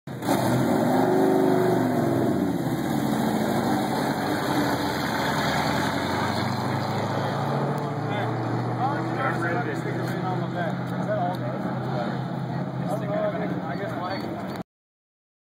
Frank Takes Off
Frank takes off out of the Pits.  He was the second to get on the track Sunday.